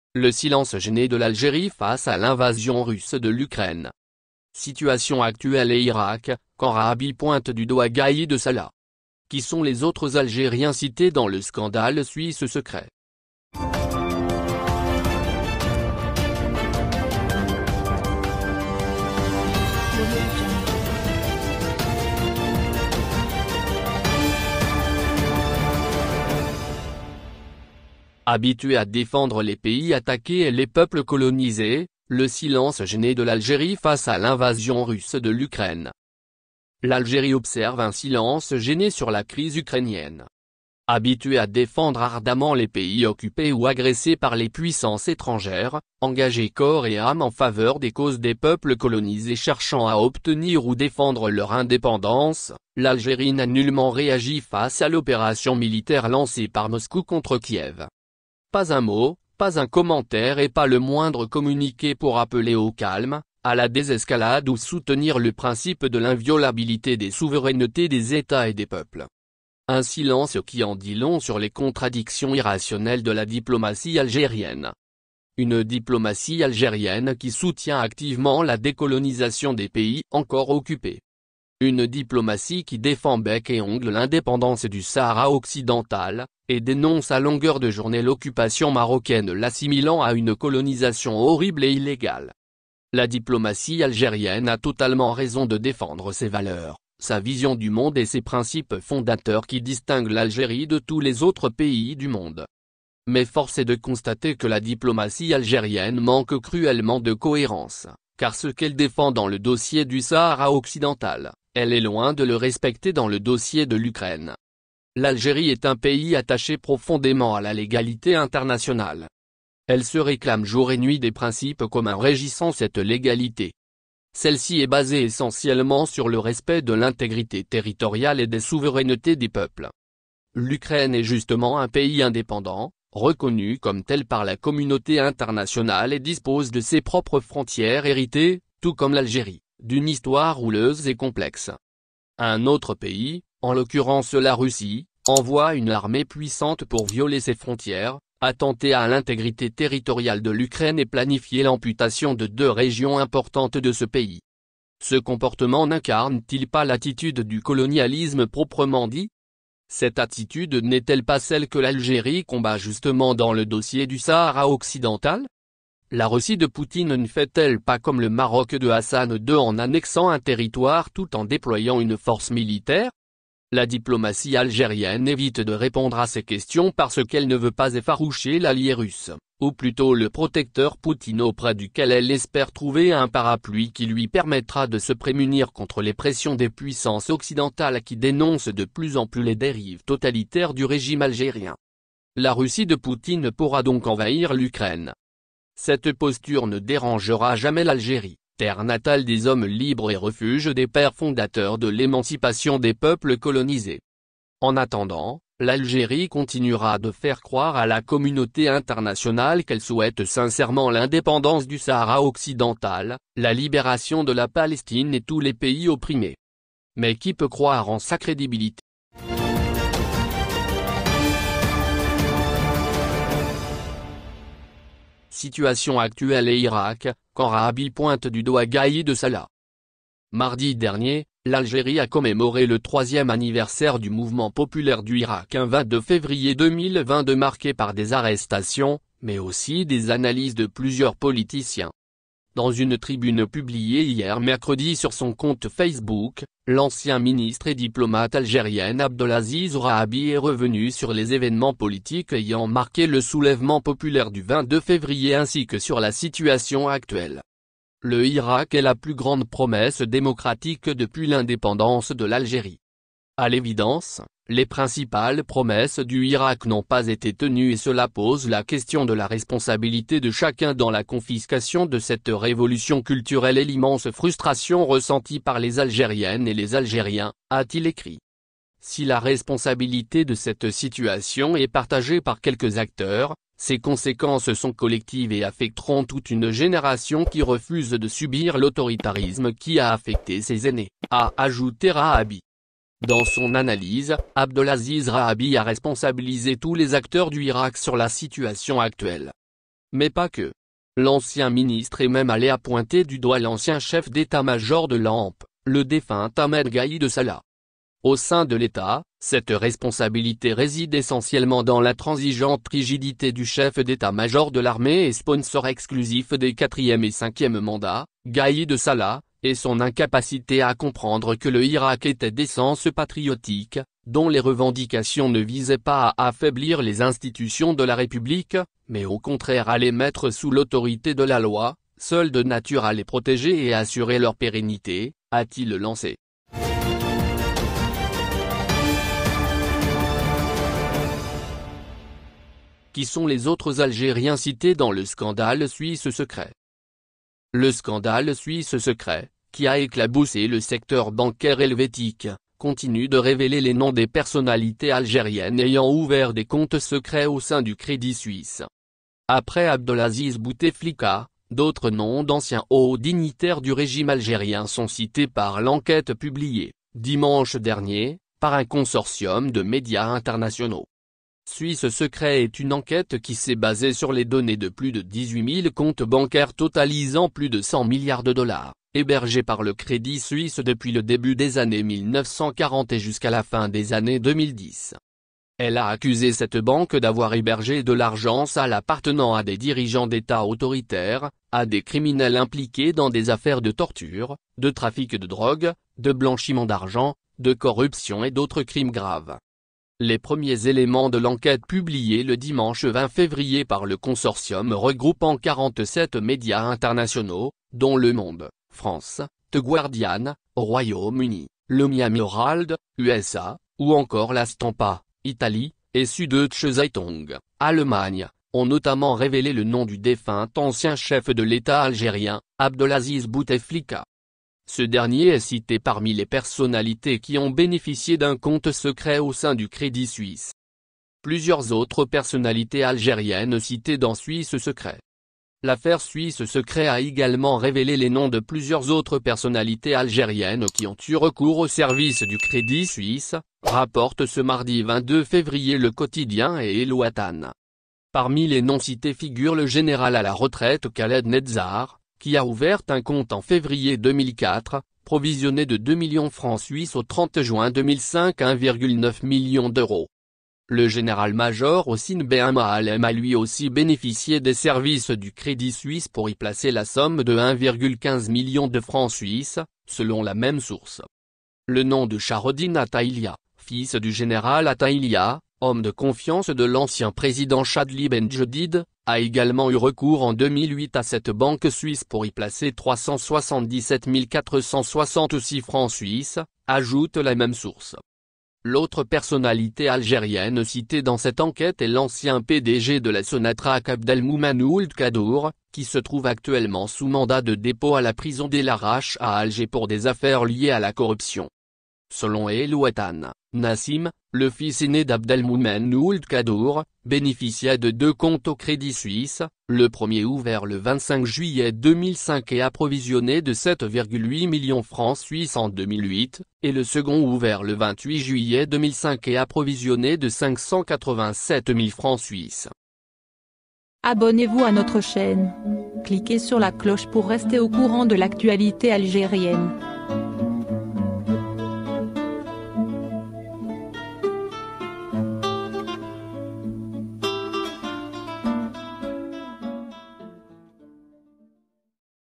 Les autres Algériens cités dans le scandale Suisse Secrets | Rahabi pointe du doigt Gaïd Salah....
L’Algérie observe un silence gêné sur la crise ukrainienne. Habituée à défendre ardemment les pays occupés ou agressés par les puissances étrangères, engagée corps et âme en faveur des causes des peuples colonisés cherchant à obtenir ou défendre leur indépendance, l’Algérie n’a nullement réagi face à l’opération militaire lancée par Moscou contre Kiev. Pas un mot, pas un commentaire et pas le moindre communiqué pour appeler au calme, à la désescalade ou soutenir le principe de l’inviolabilité des souverainetés des Etats et des Peuples. Un silence qui en dit long sur les contradictions irrationnelles de la diplomatie algérienne.
Mardi dernier, l’Algérie a commémoré le troisième anniversaire du mouvement populaire du Hirak ; un 22 février 2022 marqué par des arrestations, mais aussi des analyses de plusieurs politiciens.
Dans une tribune publiée hier mercredi sur son compte Facebook, l’ancien ministre et diplomate algérien Abdelaziz Rahabi est revenu sur les événements politiques ayant marqué le soulèvement populaire du 22 février ainsi que sur la situation actuelle.
Le scandale Suisse Secrets, qui a éclaboussé le secteur bancaire helvétique, continue de révéler les noms des personnalités algériennes ayant ouvert des comptes secrets au sein du Crédit suisse. Après Abdelaziz Bouteflika, d'autres noms d'anciens hauts dignitaires du régime algériens sont cités par l'enquête publiée, dimanche dernier, par un consortium de médias internationaux.